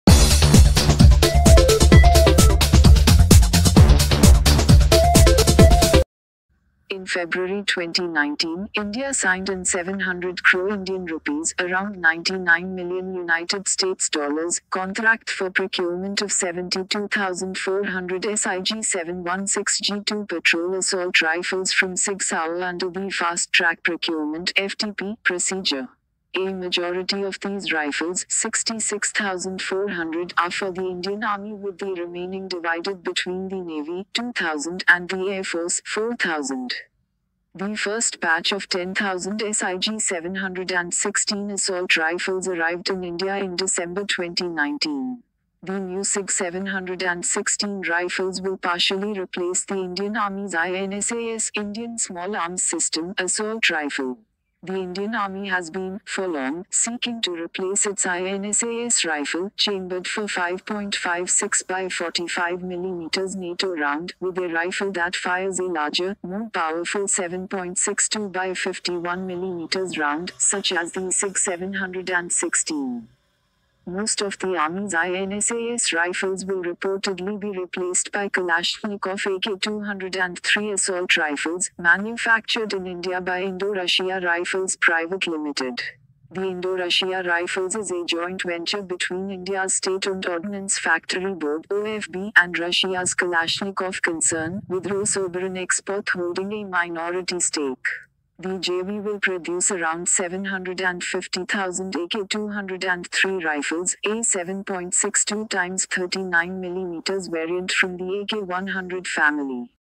Indian Army will buy 72,000 additional SIG Sauer SIG716G2 assault rifles
India signed an INR 700 crore (around USD 99 million) contract for procurement of 72,400 SIG716G2 Patrol assault rifles from Sig Sauer under the fast-track procurement (FTP) procedure. A majority of these rifles (66,400) are for the Indian Army with the remaining divided between the Navy (2,000) and the Air Force (4,000).